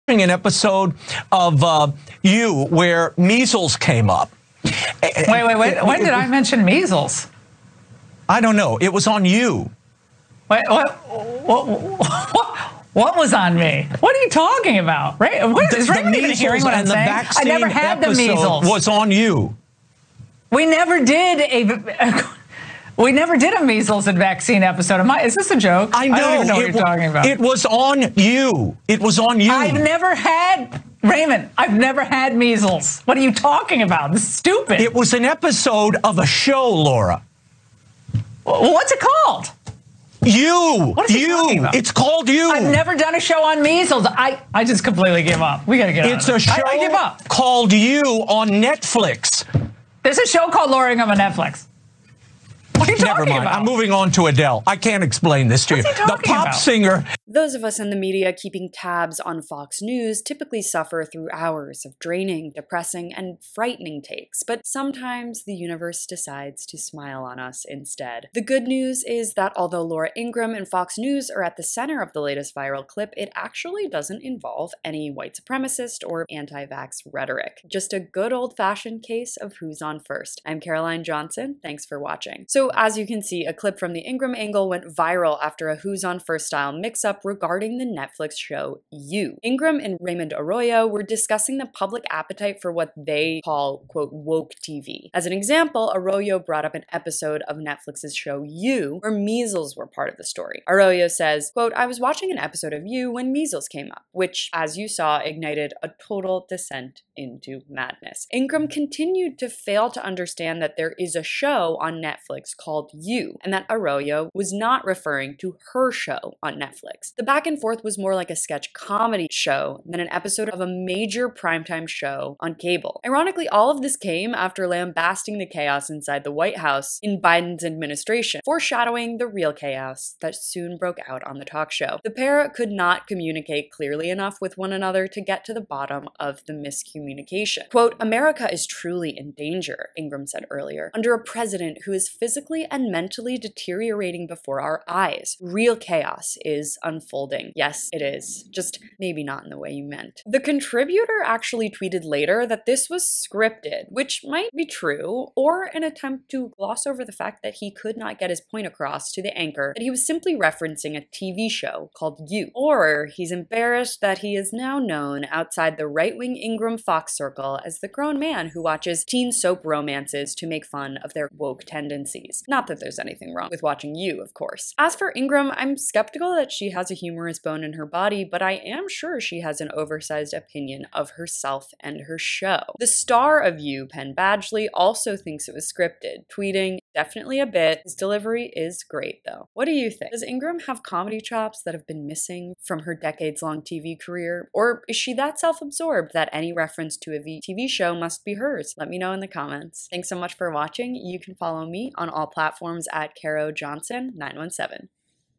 'Woke' Culture Goes Over Laura Ingraham's Head In VIRAL Video
This is on-air proof Laura Ingraham wouldn't know 'woke' culture if it hit her in the face. Caroline Johnson breaks it down on Rebel HQ.